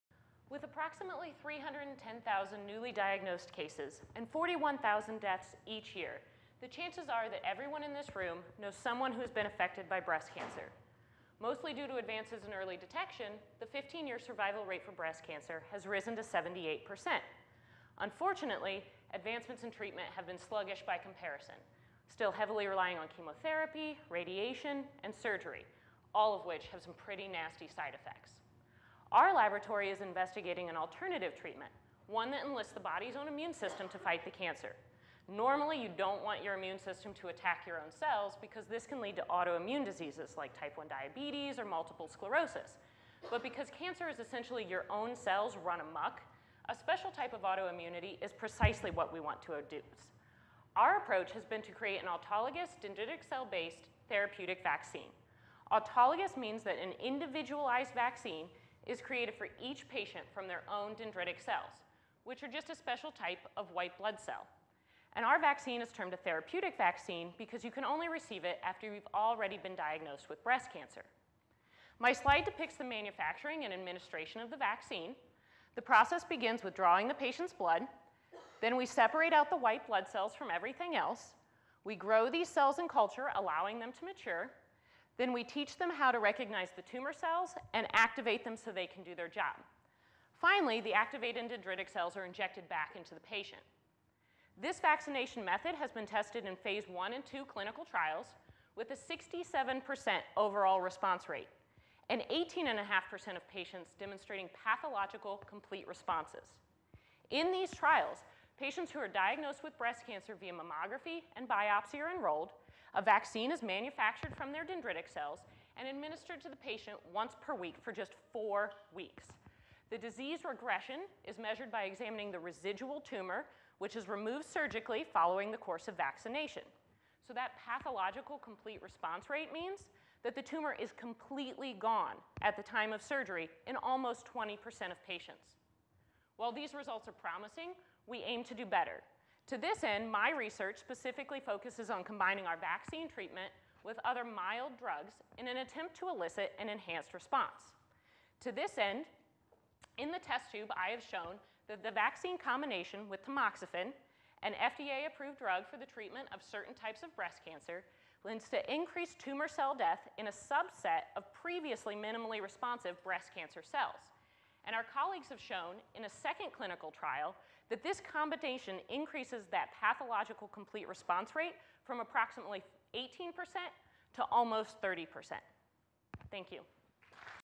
First Place 3MT Presentation: Crystal Oechsle, Kent State University
Crystal Oechsle: Has a Cure for Cancer Been Hiding Inside Us All Along? Dendritic Cell-based Vaccines for the Treatment of Breast Cancer